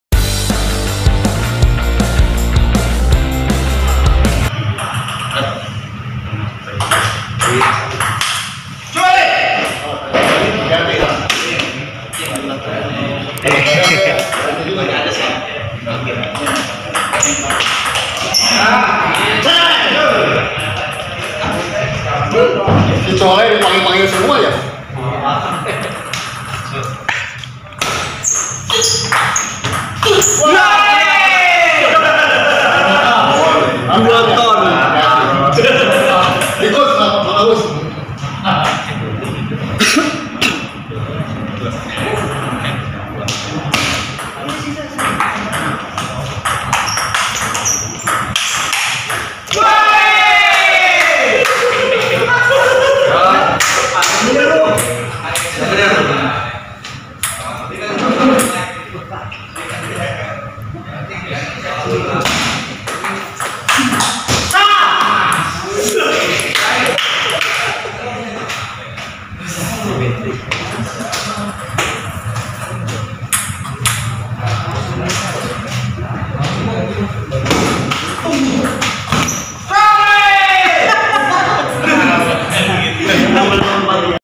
Adu Skill Tenis Meja Budi vs Anton
Sparing Ping Pong ( Tenis Meja) antara Budi dari Toyota melawan Anton Peron dari Cartic... sama sama keras bro